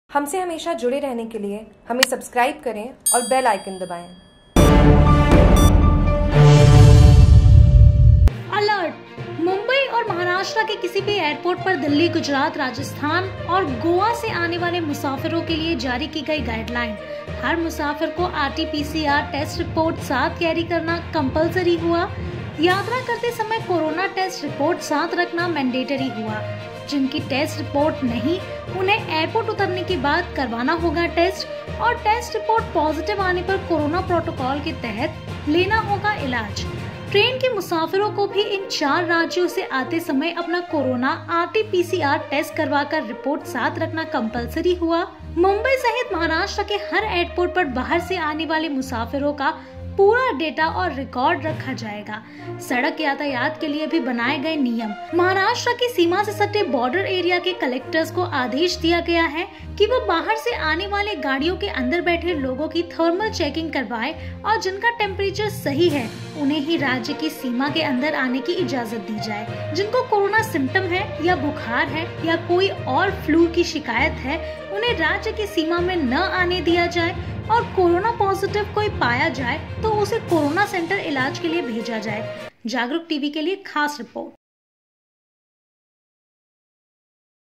Jagruk TV : अलर्ट : राजस्थान से मुंबई - महाराष्ट्र आने वालों हर प्रवासी के लिए RT-PCR जांच अनिवार्य
अलर्ट-मुम्बई और महाराष्ट्र के किसी भी एयरपोर्ट पर दिल्ली,गुजरात,राजस्थान और गोआ से आने वाले मुसाफिरों के लिए जारी की गई गाइडलाइंस

--हर मुसाफिर को RT-PCR टेस्ट रिपोर्ट साथ केरी करना कम्पलसरी हुआ

--यात्रा करते समय कोरोना टेस्ट रिपोर्ट साथ रखना मेंडेटरी हुआ

--जिनकी टेस्ट रिपोर्ट नही उन्हें एयरपोर्ट उतरने के बाद करवाना होगा टेस्ट और टेस्ट रिपोर्ट पोसिटिव आने पर कोरोना प्रोटोकॉल के तहत लेना होगा इलाज

-ट्रैन के मुसाफिरों को भी इन चार राज्यो से आते समय अपना कोरोना RT-PCR टेस्ट करवा कर रिपोर्ट साथ रखना कम्पलसरी हुआ

-मुम्बई सहित महाराष्ट्र के हर एयरपोर्ट पर बाहर से आने वाले मुसाफिरों का पूरा डेटा और रिकॉर्ड रखा जाएगा

-सड़क यातायात के लिए भी बनाये गए नियम

-महाराष्ट्र की सीमा से सटे बॉर्डर एरिया के कलेक्टर्स को आदेश दिया गया है कि वो बाहर से आने वाली गाड़ियों के अंदर बैठे लोगों की थर्मल चेकिंग करवाये और जिनका टेम्परेचर सही है उन्हें ही राज्य की सीमा के अंदर आने की इजाज़त दी जाए

-जिनको कोरोना सिम्पटम है या बुखार है या कोई और फ्लू की शिकायत है उन्हें राज्य की सीमा में न आने दिया जाए और कोरोना पोसिटिव कोई पाया जाए तो उसे कोरोना सेंटर इलाज के लिए भेजा जाए